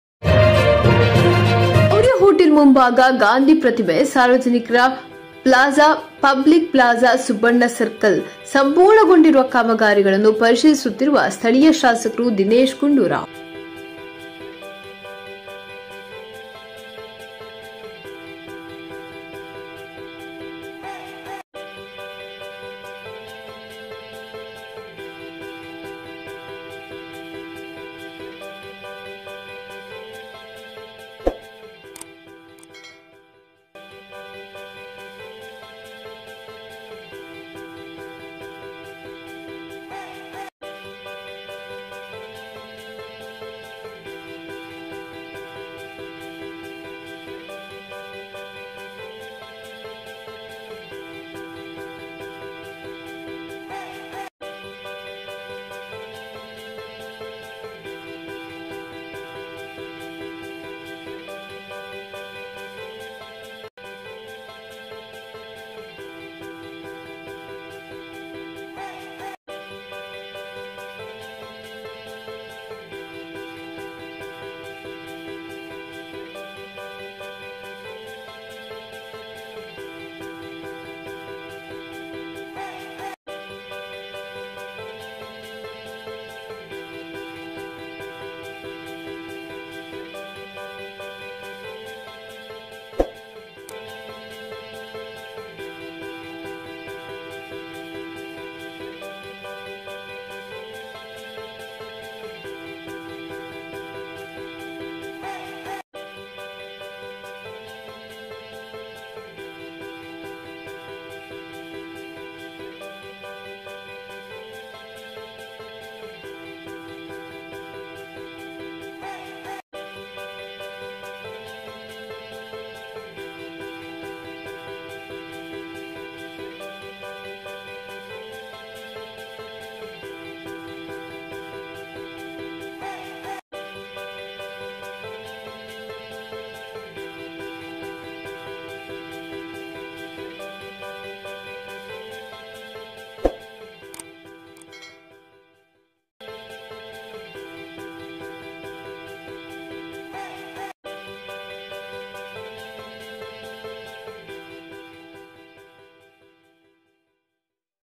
ಸಾರ್ವಜನಿಕರ ಪ್ಲಾಜ ಸುಬ್ಬಣ್ಣ ಸರ್ಕಲ್ ಸಂಪೂರ್ಣಗೊಂಡಿರುವ ಕಾಮಗರಿ ಪರಿಶೀಲಿಸುತ್ತಿರುವ ಶಾಸಕರು ದಿನೇಶ್ ಗುಂಡೂರಾವ್
ಮೌರ್ಯ ಹೋಟೆಲ್ ಮುಂಬಾಗ ಗಾಂಧಿ ಪ್ರತಿಮೆ ( ಸಾರ್ವಜನಿಕರ ಪ್ಲಾಜ public plaza) ಸುಬ್ಬಣ್ಣ ಸರ್ಕಲ್ ಸಂಪೂರ್ಣಗೊಂಡಿರುವ ಕಾಮಗರಿಗಳನ್ನು ಪರಿಶೀಲಿಸುತ್ತಿರುವ ಸ್ಥಳೀಯ ಶಾಸಕರು ದಿನೇಶ್ ಗುಂಡೂರಾವ್